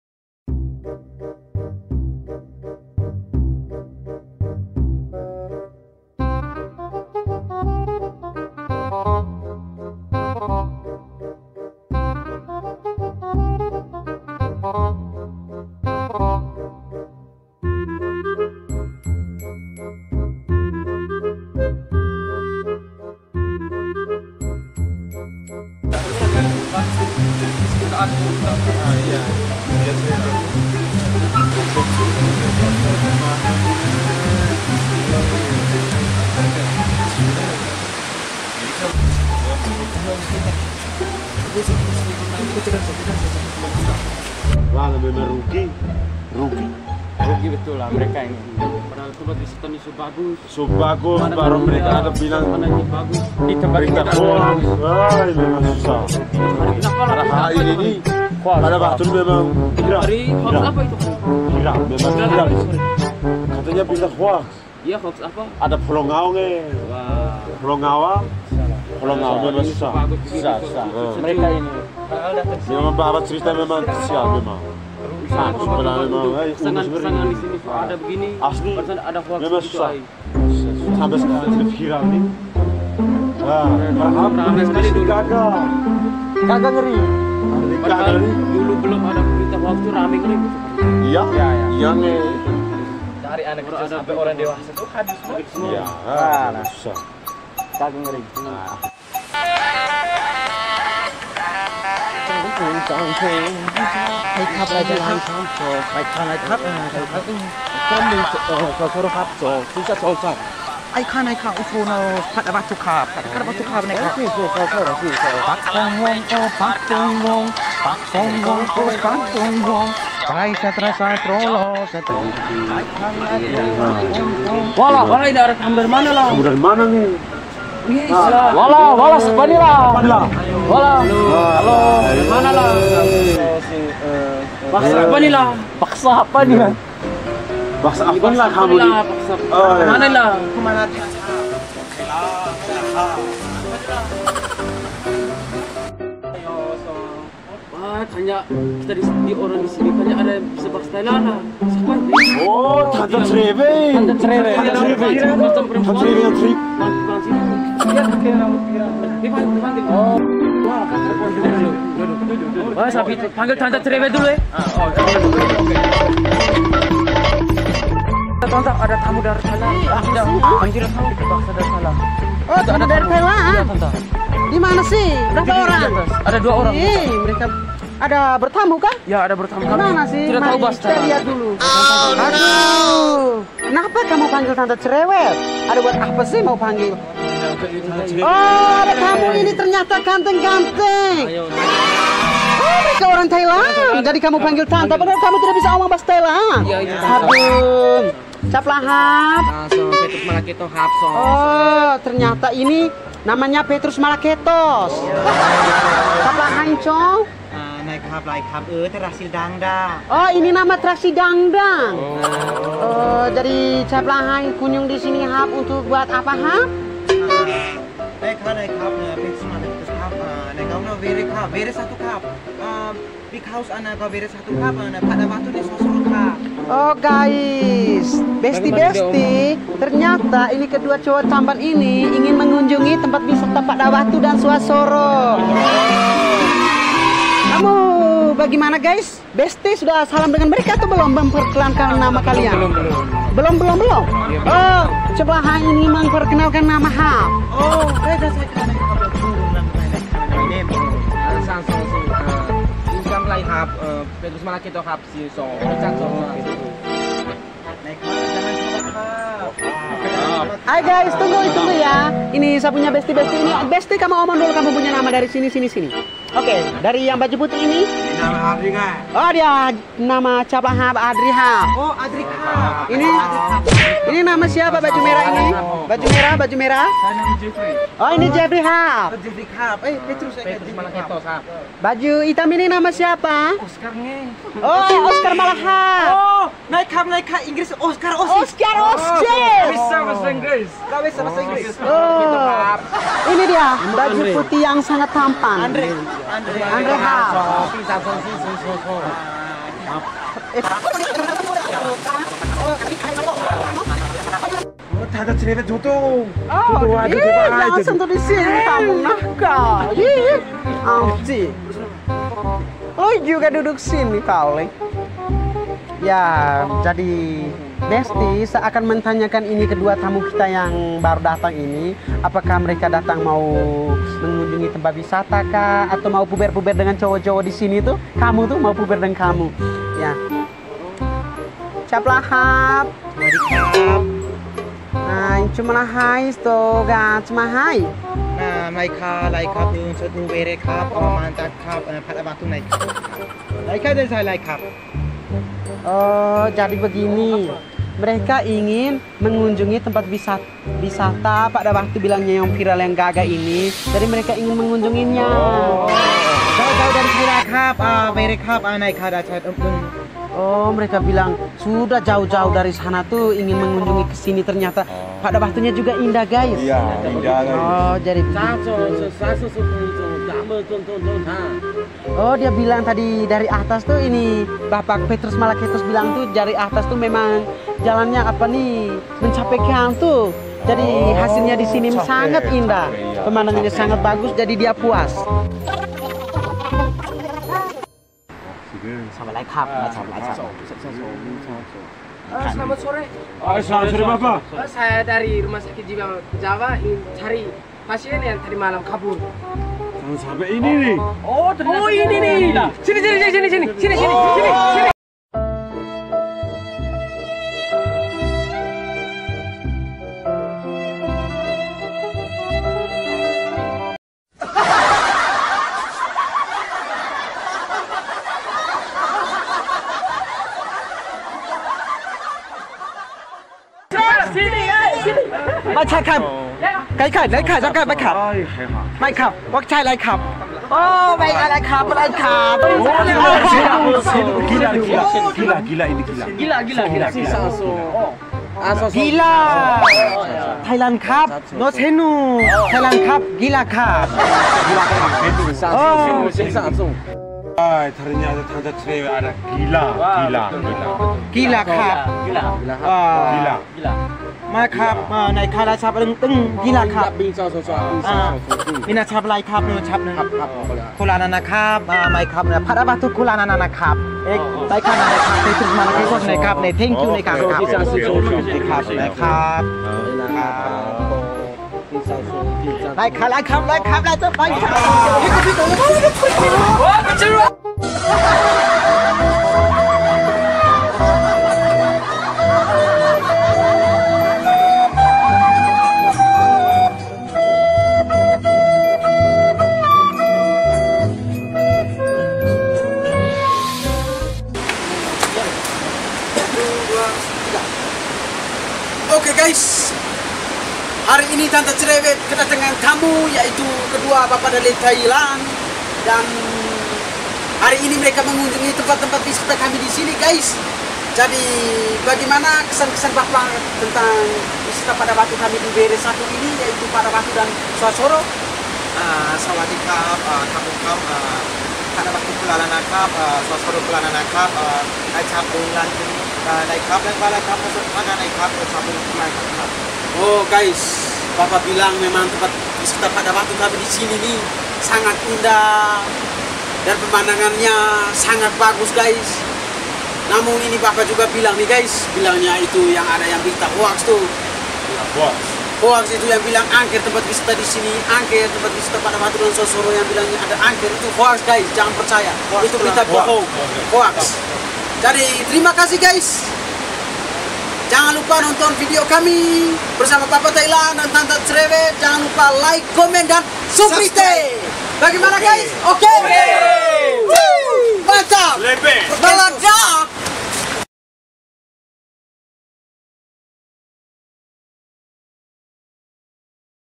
FILM PENDEK KOMEDY BAJAWA 2023🤣 "Kunjungan Ke Padha Watu"//Tanta Cerewet VS Bapak Thailand...
Terimakasih Buat teman-teman semuanya sudah Kunjung Ke Channel Ini...😁